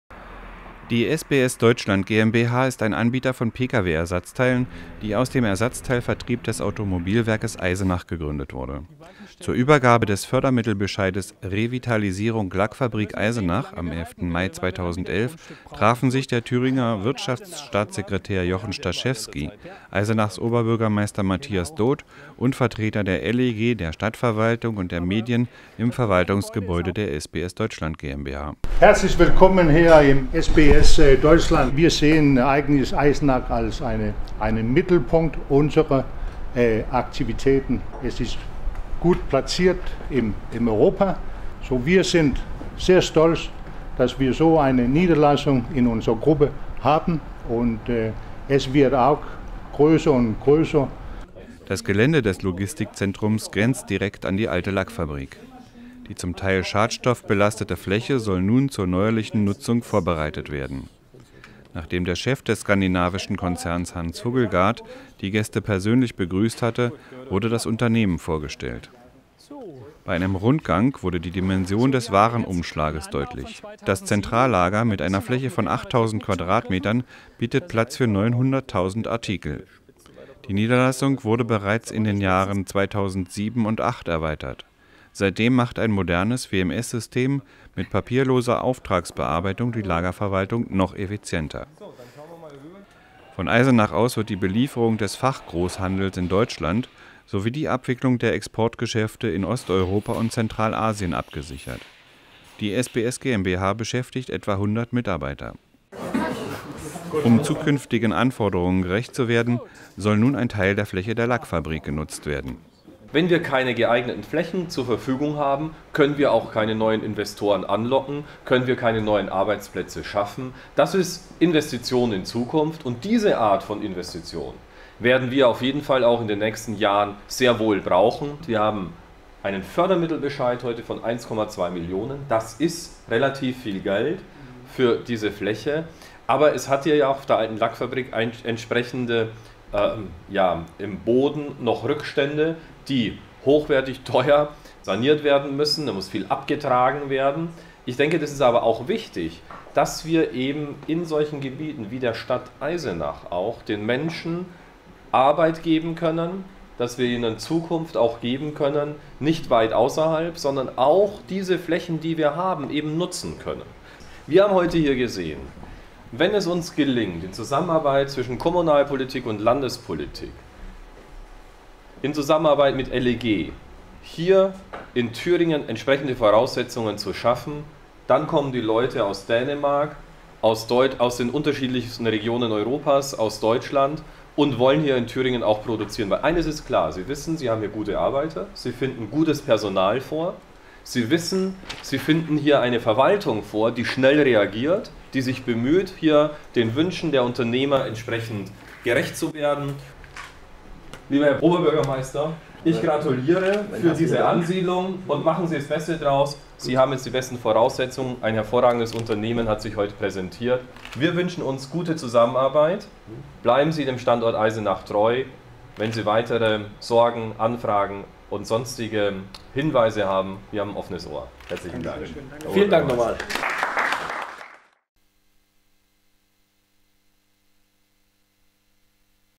Wirtschaftsnachrichten Eisenach
Bericht über die Übergabe des Fördermittelbescheides an die Stadt zur Revitalisierung der Lackfabrik am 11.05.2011 bei SBS Deutschland